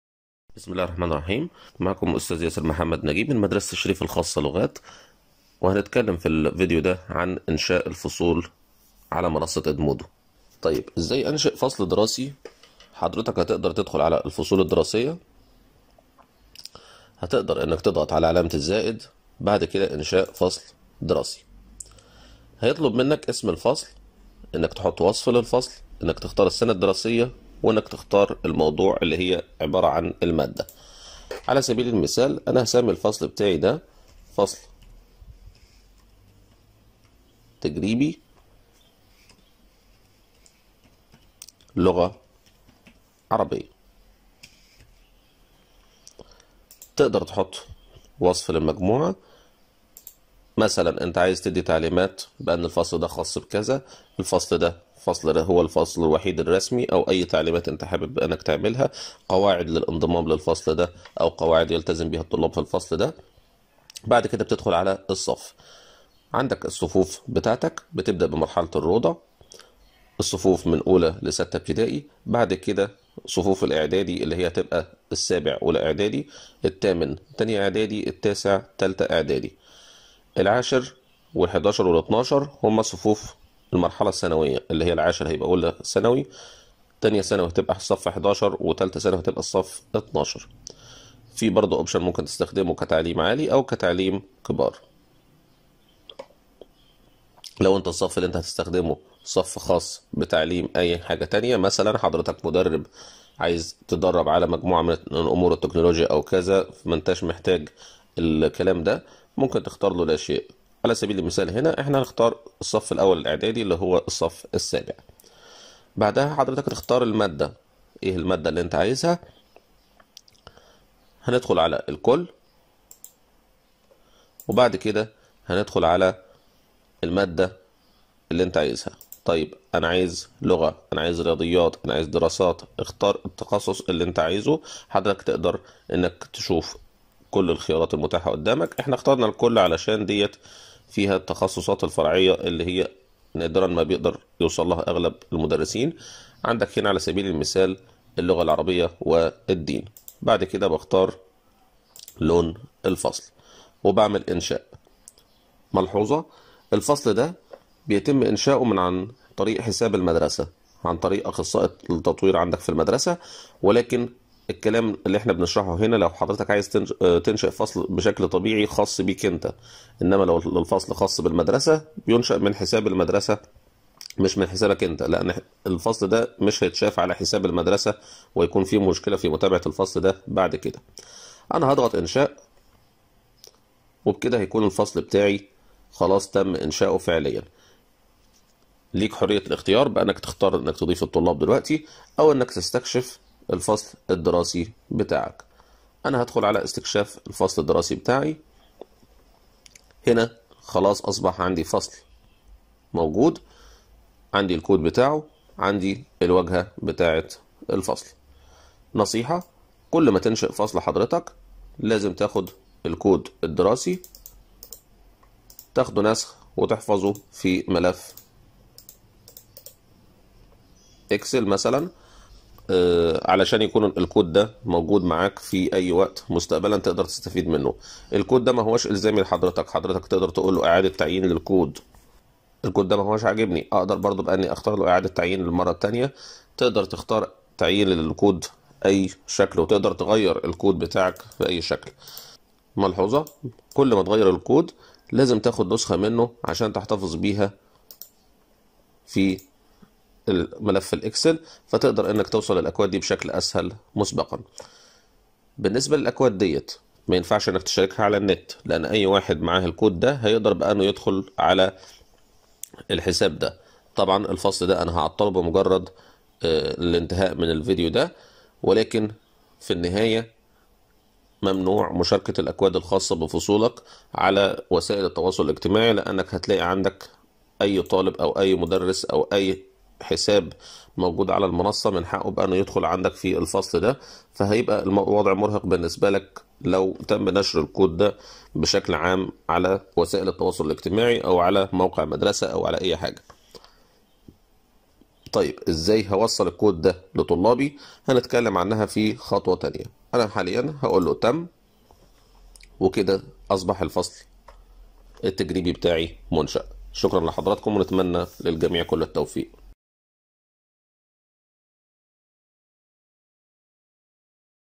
Edmodo - حساب المعلم (4) - إنشاء الفصول الافتراضية
في هذا الفيديو نعرض لكم :
- كيفية إنشاء فصل دراسي .
- استكشاف الفصل الدراسي .
- كود الفصل الدراسي .

وهو الفيديو الرابع من سلسلة شرح حساب المعلم على منصة إدمودو التعليمية

وذلك ضمن سلسلة تستهدف نشر التوعية بالمنصة وكيفية استخدامها الاستخدام الأمثل .. والتعريف بكل أدوات التعلم على المنصة ..

صممت هذه المادة لإفادة أعضاء هيئة التدريس بمدرسة الشريف الخاصة لغات
ولجميع السادة المعلمين في أنحاء البلاد

سائلين المولى عز وجل دوام التوفيق والنجاح للجميع